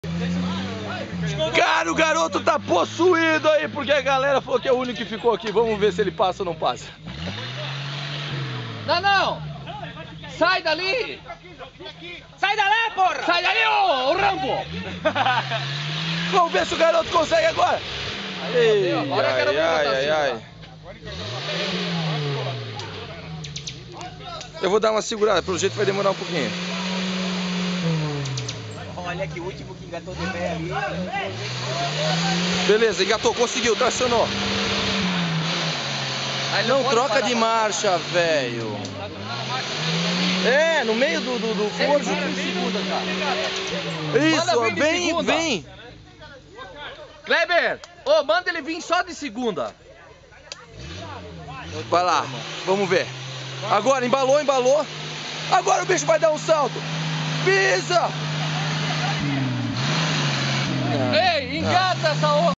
Trilhão Jeep Club Jaraguá do Sul 2011 - 12
Imagens e narração do já embriagado Luis.